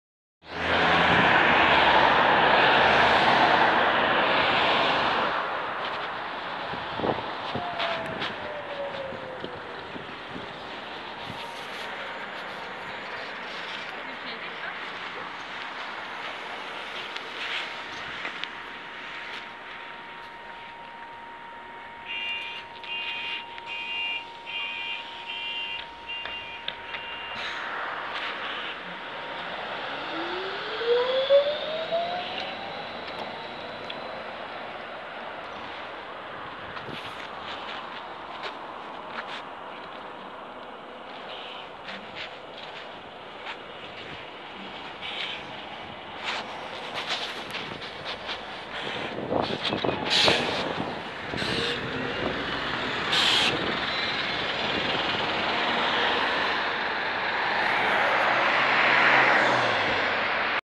Trolejbus URSUS CS18T #3953 oraz Autobus Jelcz M121I/4 #2517
Polskie Wozy:
❤TROLEJBUS Ursus City Smile18T
MPK Lublin #3953 Linia 151 ABRAMOWICE

AUTOBUS Jelcz M121I/4 Mastero (ex. #4941)
MPK Lublin #2517 Linia 25 PRAWIEDNIKI